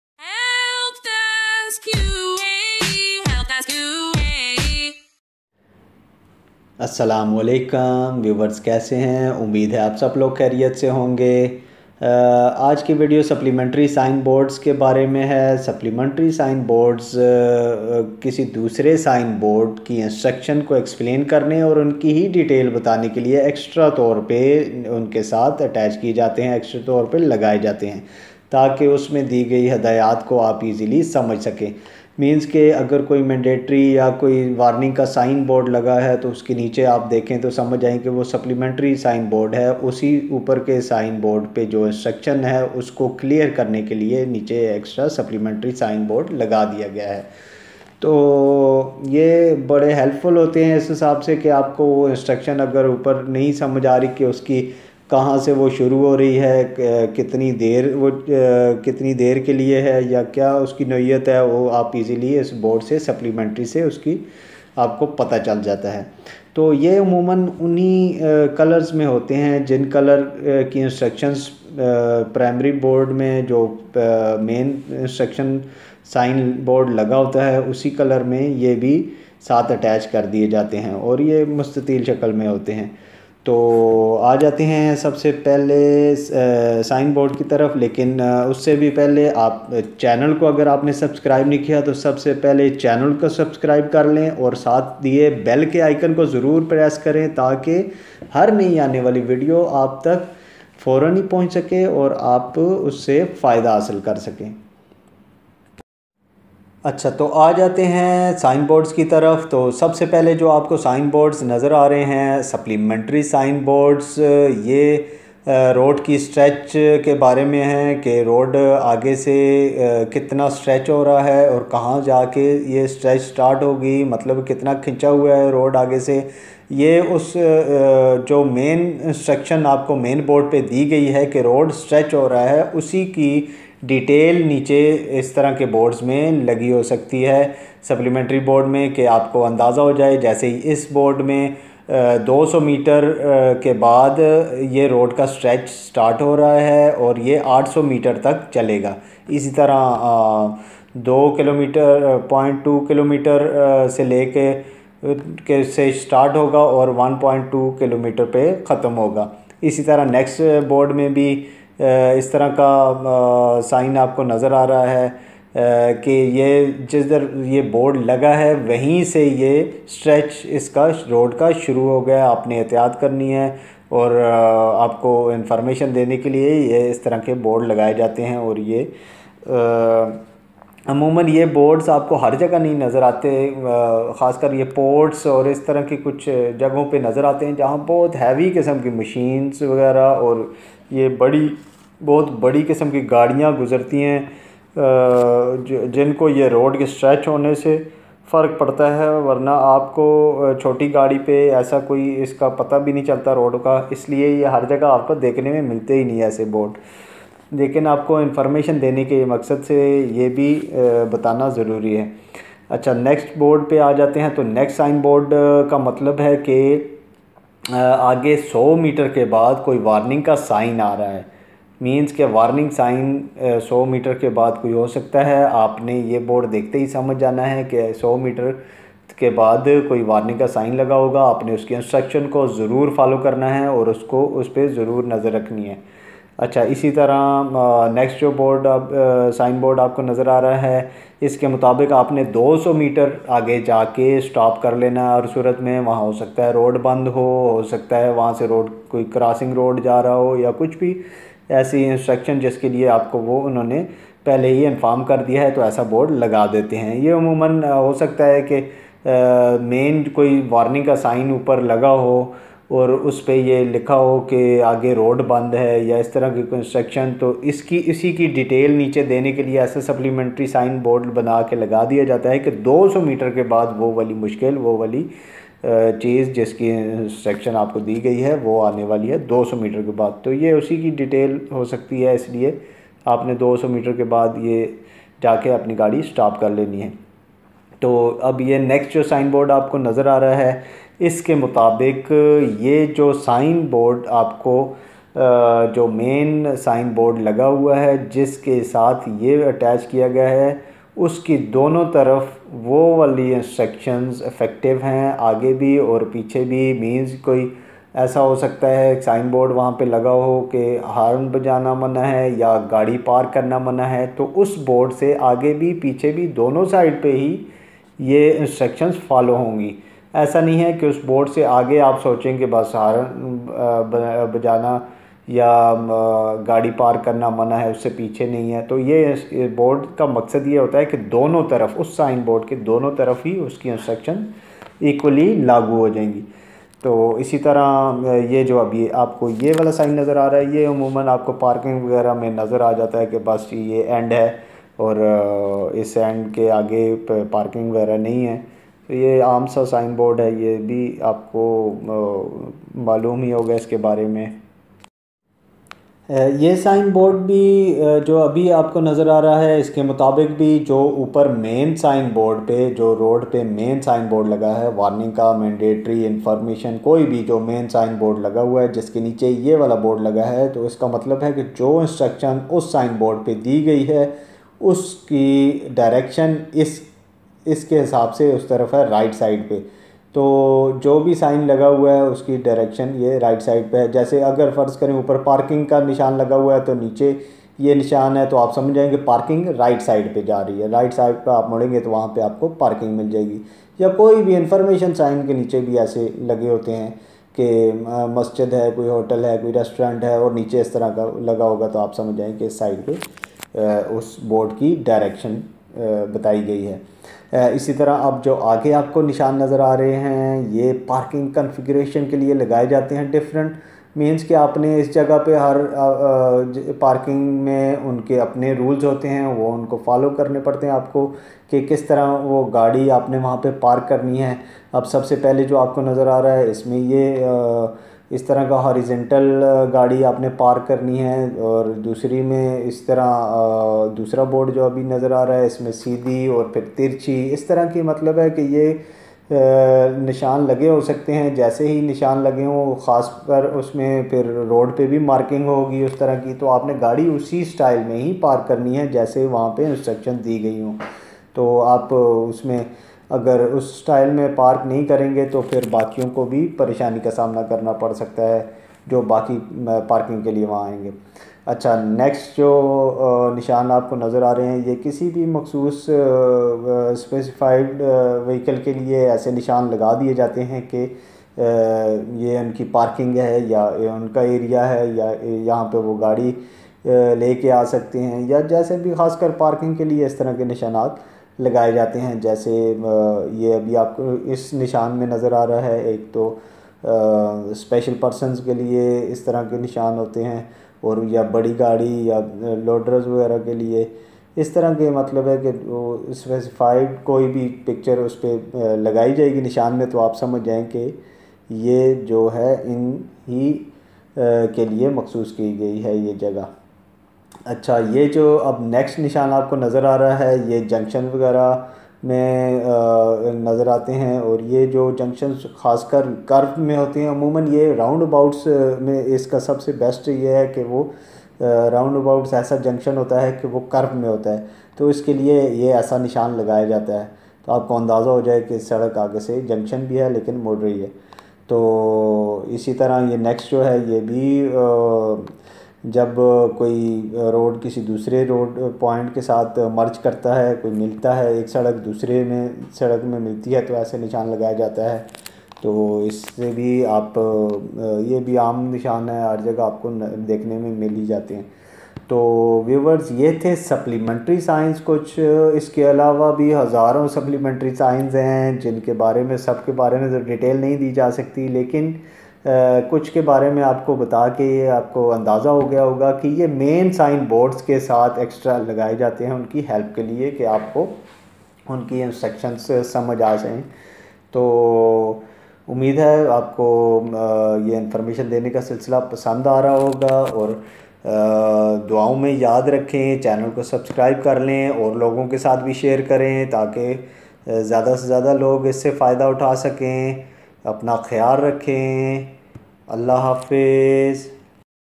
Road Safety Signs | Supplementary Board | Driving Road Rules | Urdu | Hindi
Road Safety Signs | Supplementary Board | Driving Road Rules | Urdu | Hindi                              Road Sign Boards & Signals /Supplementary/Driving Rules in driving license.                                  In this video you can know about supplementary road sign boards in Hindi and Urdu.                   Its helpful information about road signs ,watch full video of supplementary road sign it will help if you drive or taking driving license. driving rules are very important and sign and signal boards are must in driving . thanks

is video main aap jan saken ge kuch Sign boards aor signals ke bare main take aap ke driving license lene main bhe asani ho aor doran drive kisi kisam ka jurmana/fine ya mukhalfa na dena pare